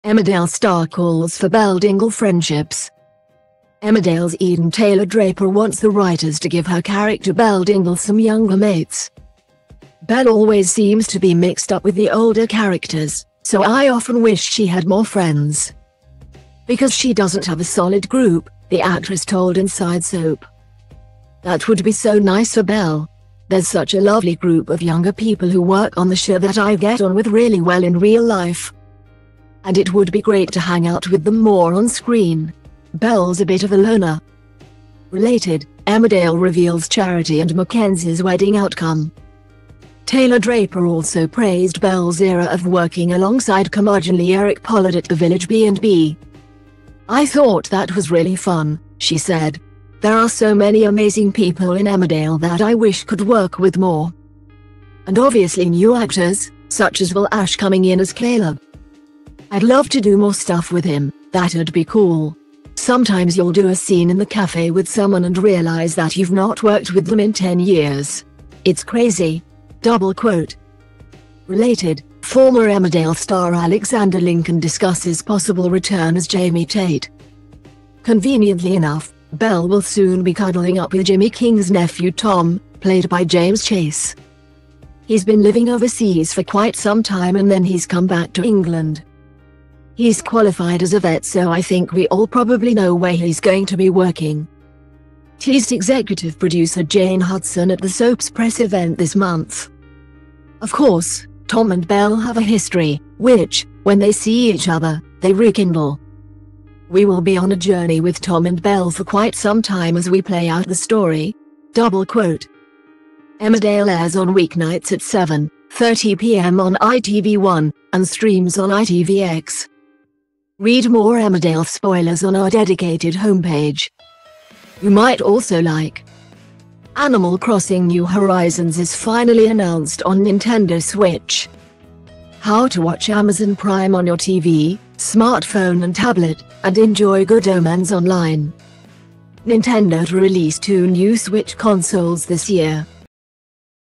Emmerdale star calls for Belle Dingle friendships
Emmerdale's Eden Taylor-Draper wants the writers to give her character Belle Dingle some younger mates. "Belle always seems to be mixed up with the older characters, so I often wish she had more friends,because she doesn't have a solid group, " the actress told Inside Soap.